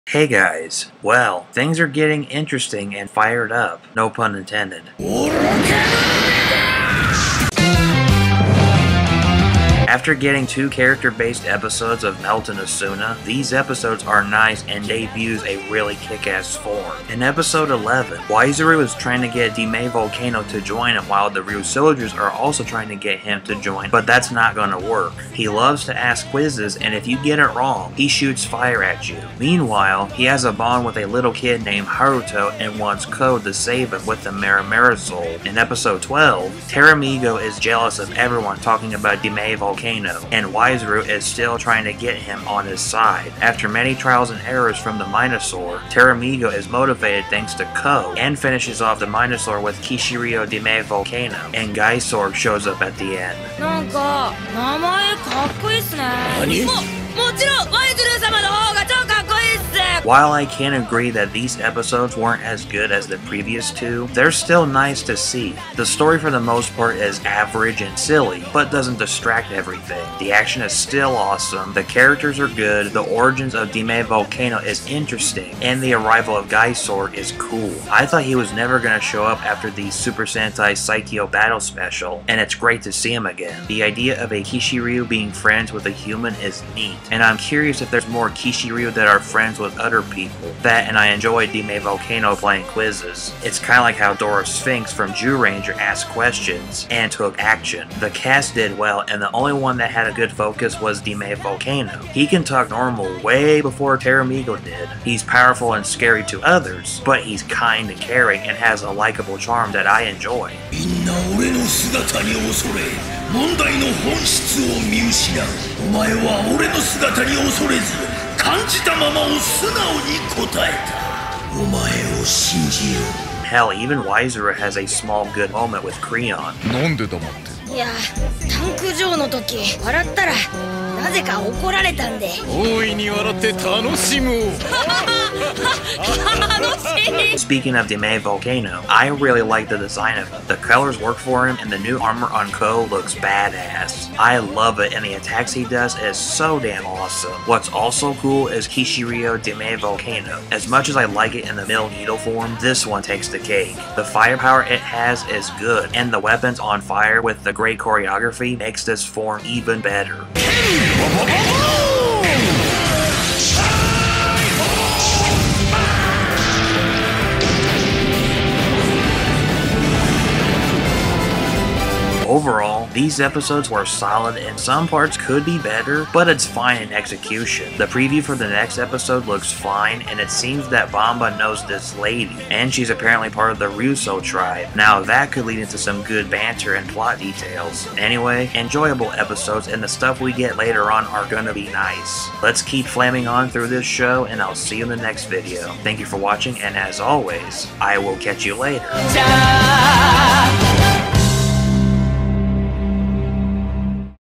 My Thoughts on Kishiryuu Sentai Ryusoulger (Episode 11-12)
We've been through some character development, saw some cool Minosaurs and saw the fully combined Kishiryuu-Oh Five Knights.

Here, we get a debut of a new Kishiryuu and a new form for the Ryusoulgers. What do I think about these episodes? Were they good or decent at best? Well, let's flame on while answering quizzes and find out!